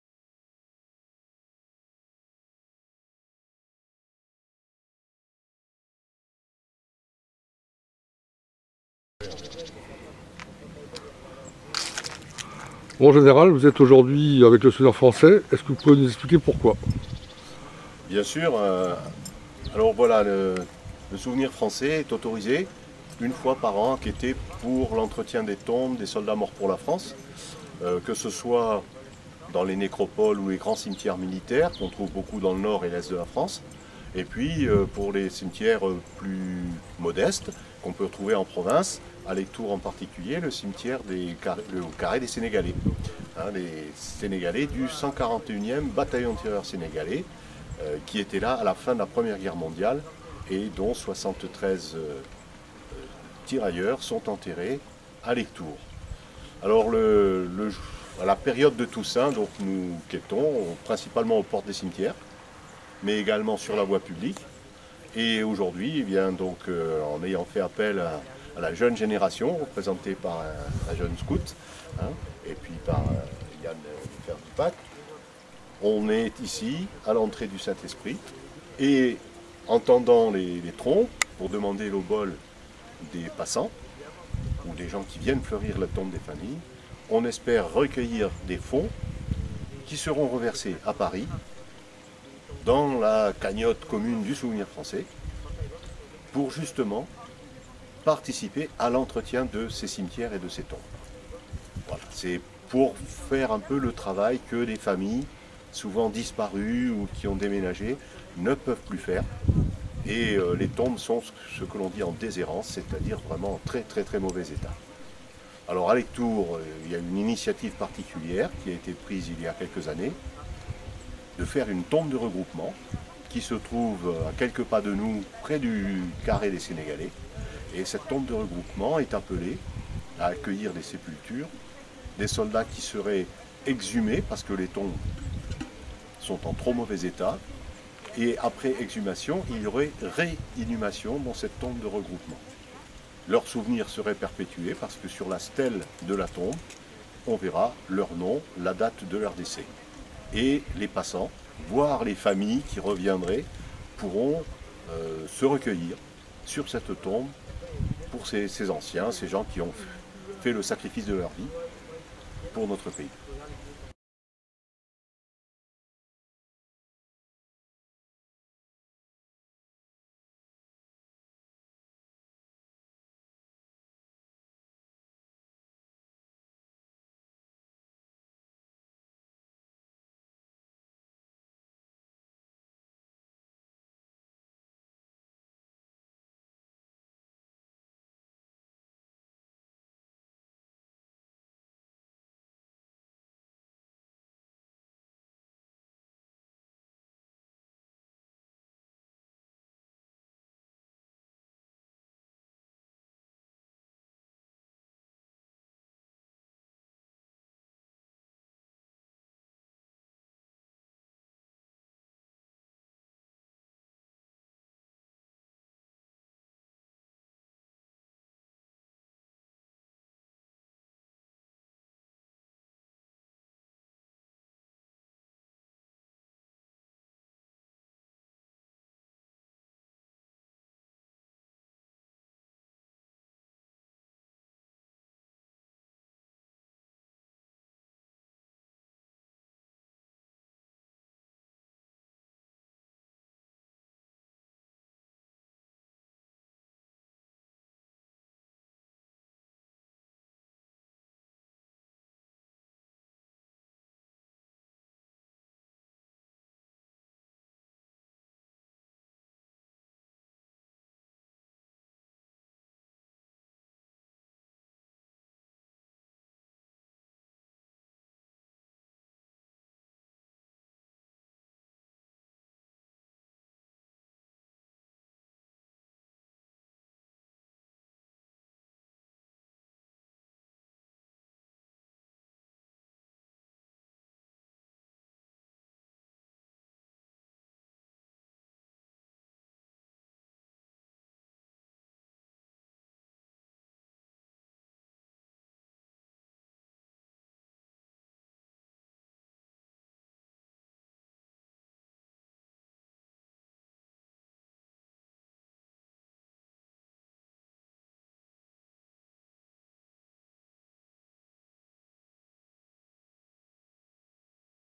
comité de Lomagne du Souvenir français
interview du Général Eric Boss, président du comité de Lomagne du Souvenir français, au sujet de la quête afin d'entretenir les tombes en déshérence des soldats morts pour la France (MPLF)